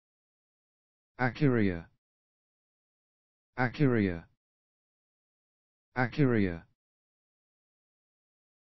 How to Pronounce acheiria
How to Pronounce acheiria with UK
English Phonetic UK [eɪ'keərɪə]